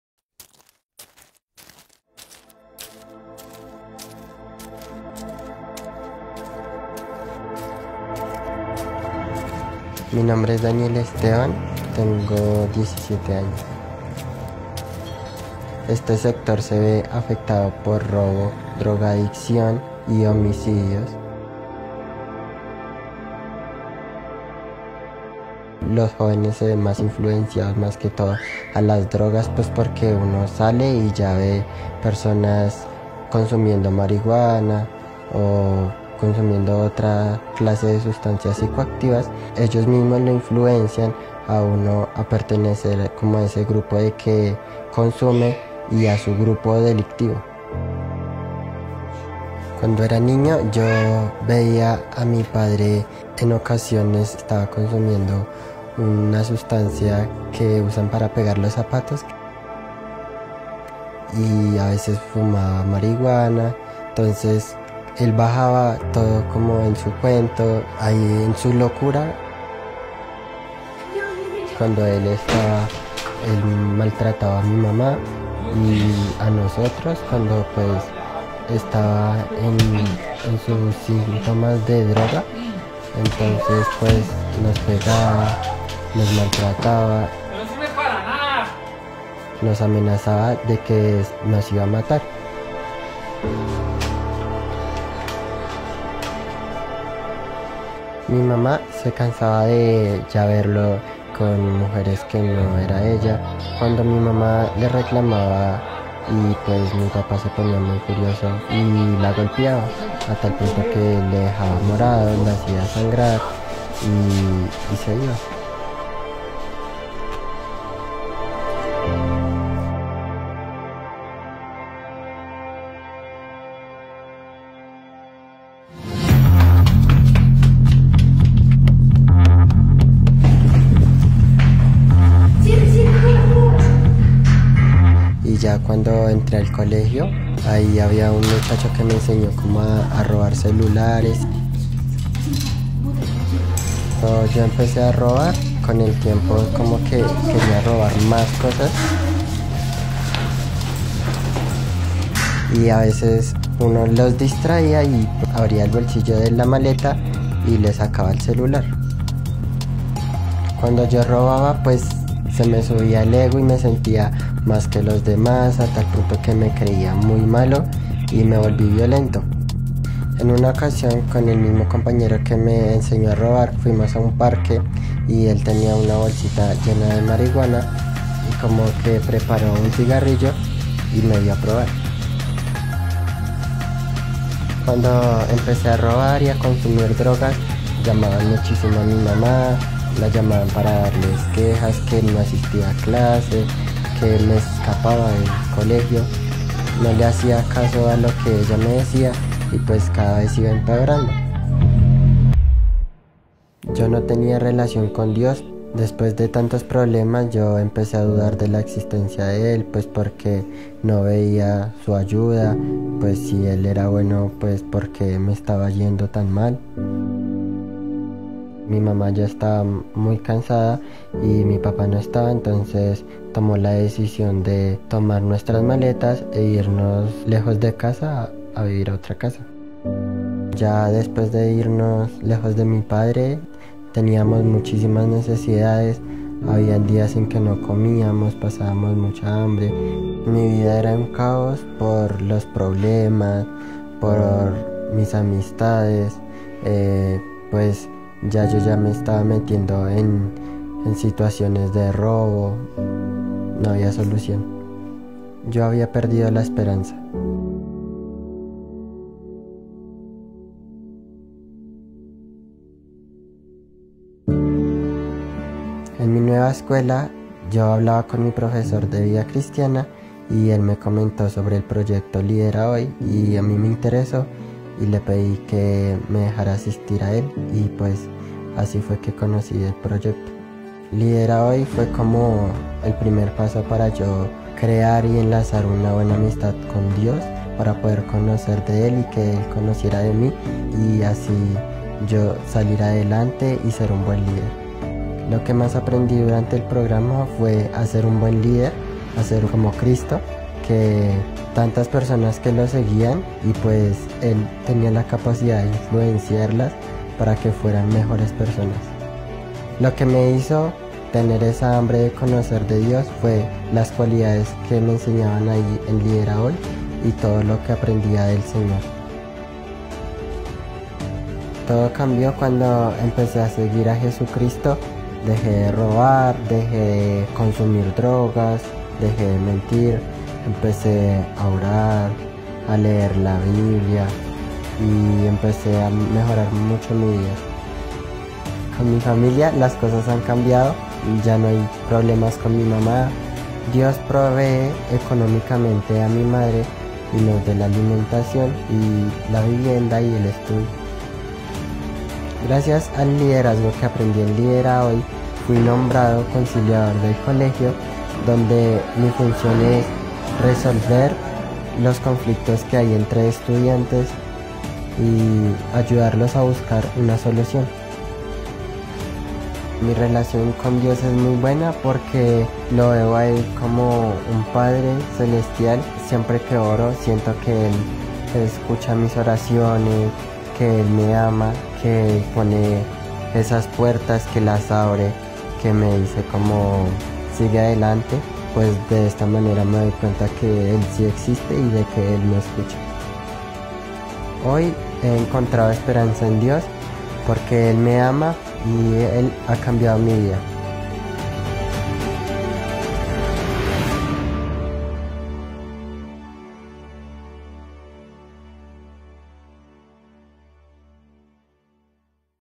Testimonio de impacto del programa LIDERAHOY.
La mayoría de las personas no lideran sus vidas, solamente aceptan su vida.
LIDERAHOY es un programa de liderazgo para adolescentes y jóvenes.